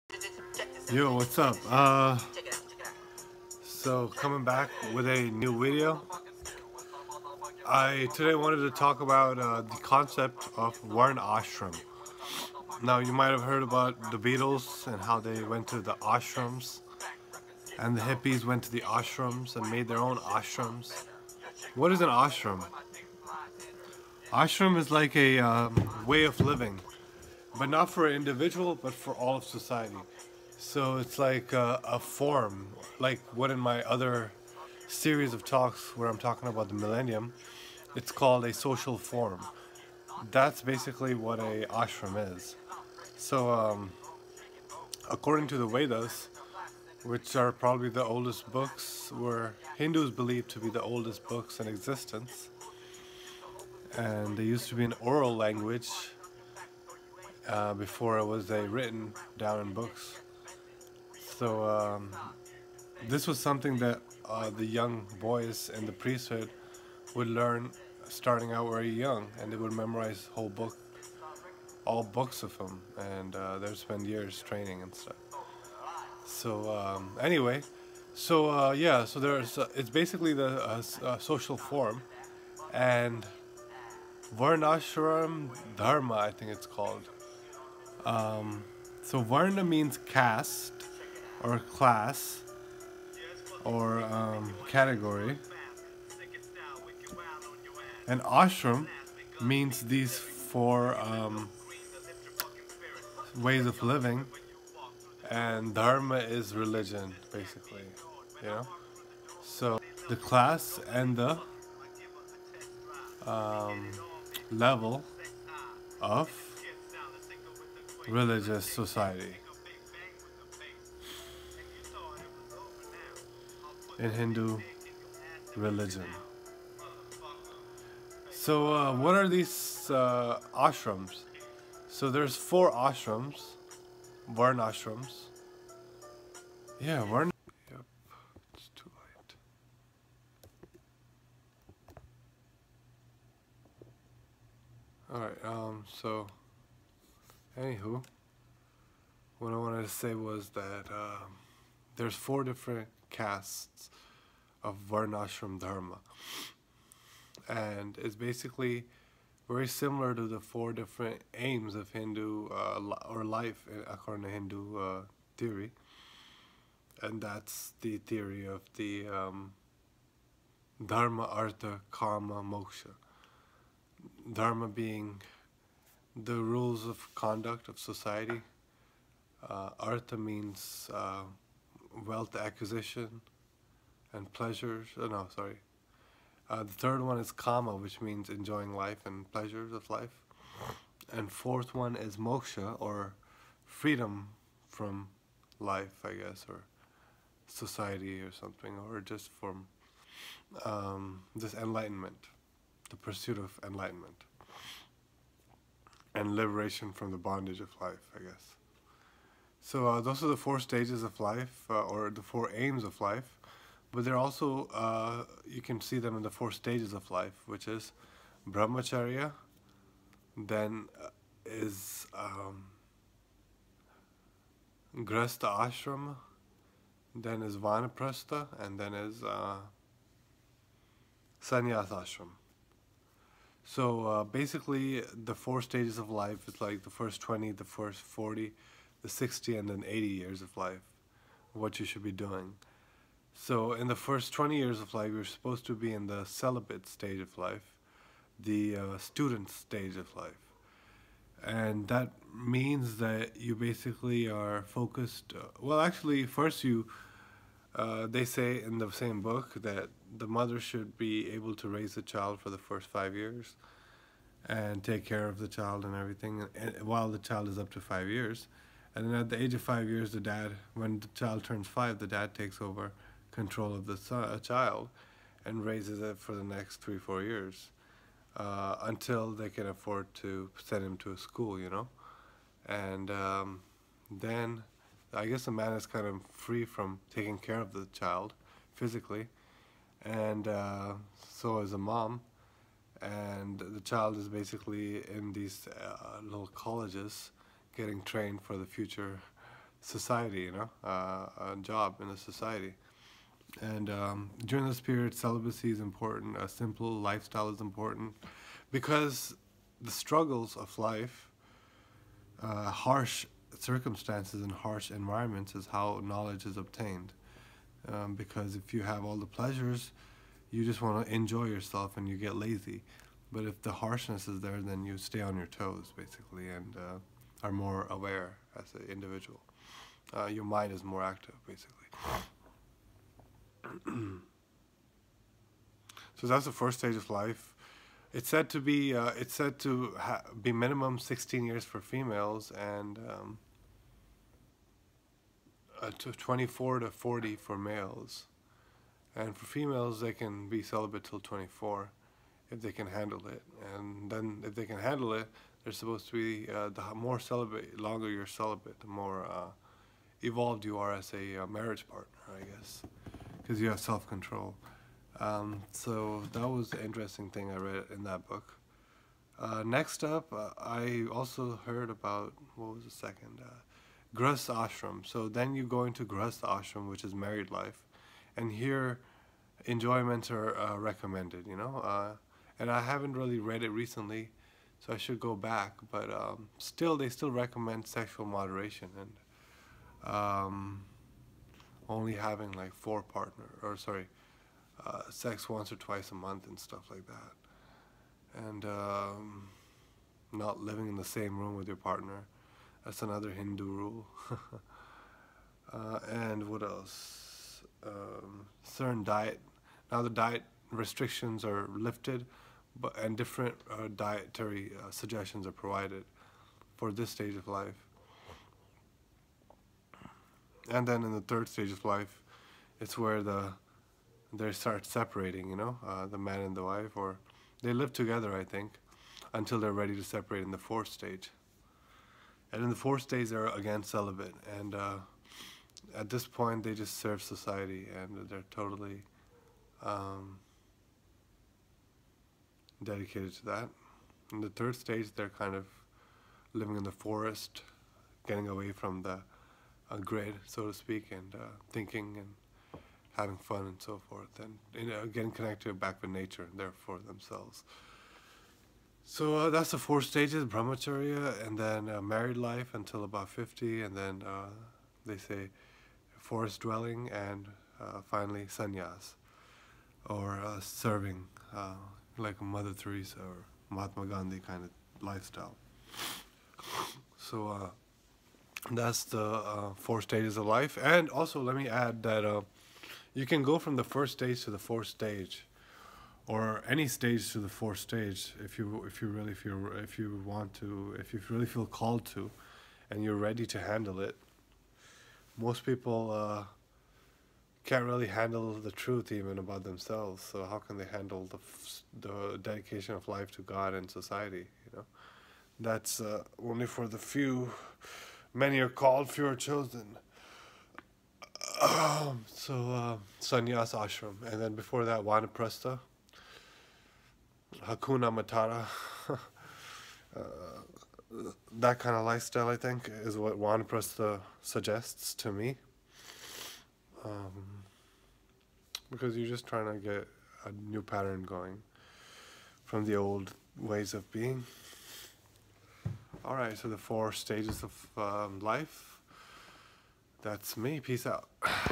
The 4 Orders & Stages of Life (varna ashram dharma)
Orders/Castes: Brahmin/Priest, Kshatriyas/King, Vaishya/Merchants and Shudra (slave/commoner).
Stages: Celibate Student (Brahmacharya), Married (Grihastha), Retired (Vanaprastha), Ascetic/Saint.

Image borrowed from following website on topic (no endorsement from me to this particular site)